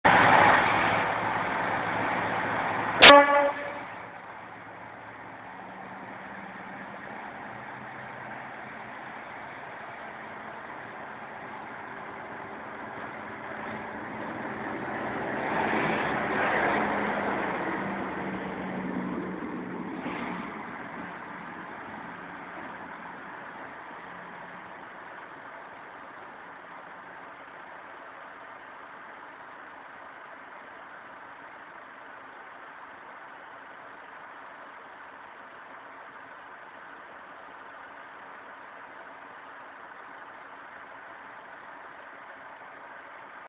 56302 Leeds
56302 takes the road home 0Z57 after a crew training run to Shipley.
14.2.12